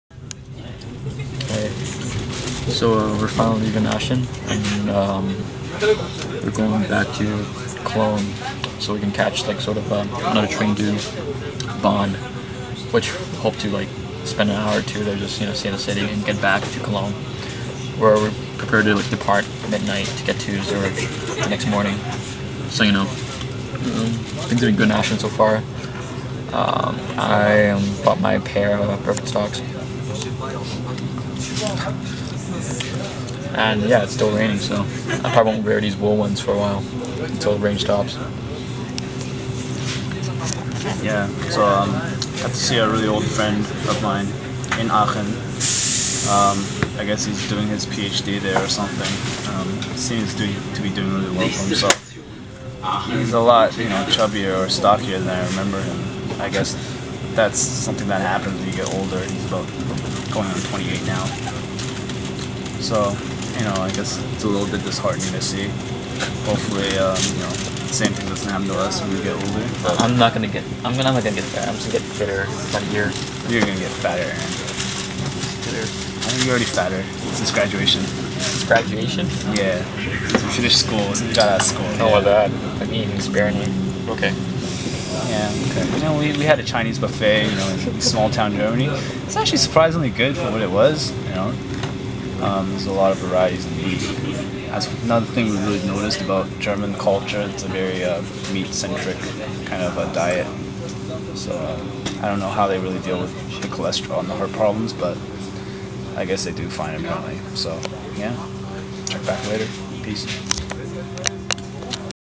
6. Train of thoughts
Hours of trains through non-descript locations can lead to occasional bouts of randomness.  Here was one such random stream of thoughts on our journey.